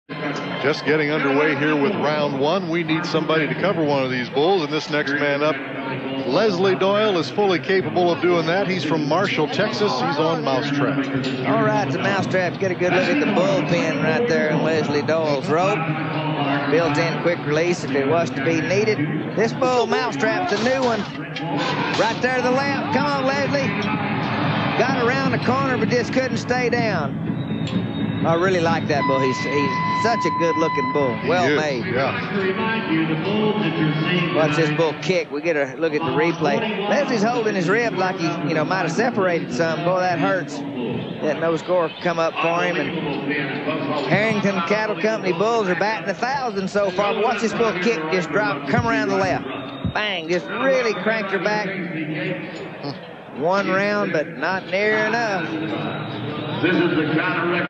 Mouse Trap bucks Leslie Doyle - 00 PBR Ft. Worth
Brandon Gasper & Chad Herrington's disliked eliminator, 0X3 Mousetrap loosens Leslie Doyle off with the hop and skip during Round 1 of the 2000 Tuff Hedeman Challenge in Fort Worth, TX. Nobody could figure this bull out. Bull score: 45.50